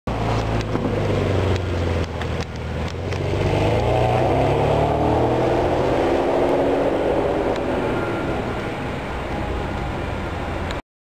Ferrari 599 GTB Fiorano Sound - Rome
Me filming this 599 GTB going into the Sa.Mo.Car. There was Nonsolonoia too.